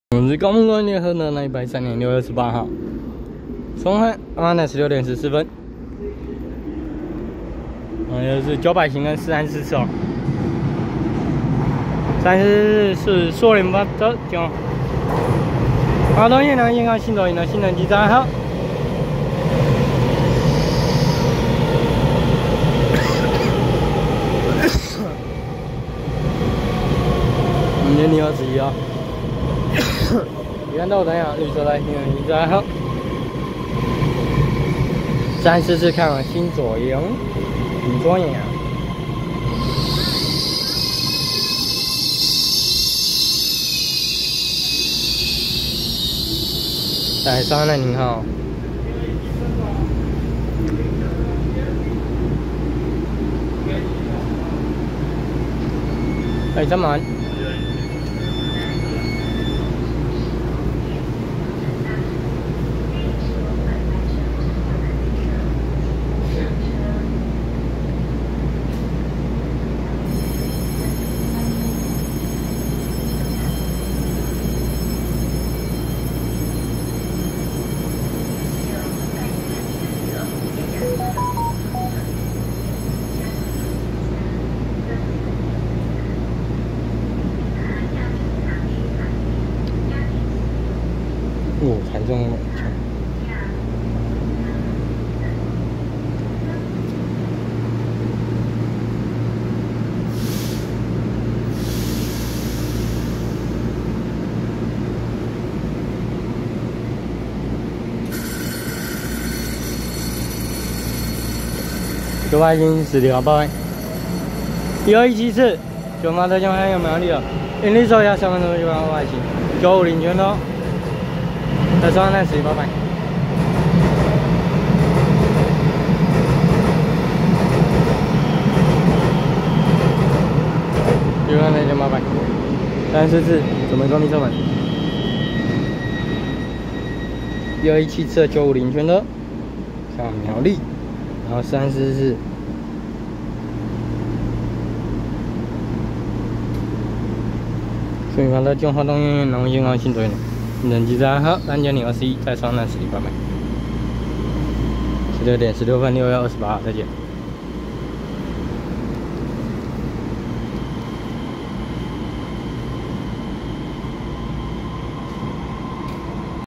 434次樹林發車開往新左營的EMU3000型十二輛編組EMU3021皮蛋豆腐騰雲號綠色帶新城際自強號列車，1217次基隆發車經由山線開往苗栗的銀綠色微笑號EMU900型十輛一編組EMU950區間車
113/6/28松山站16:14分拍攝434次樹林發車經由南迴線開往新左營的EMU3000型十二輛編組EMU3021皮蛋豆腐騰雲號綠色帶新城際自強號列車，1217次基隆發車經由山線開往苗栗的銀綠色微笑號EMU900型十輛一編組EMU950區間車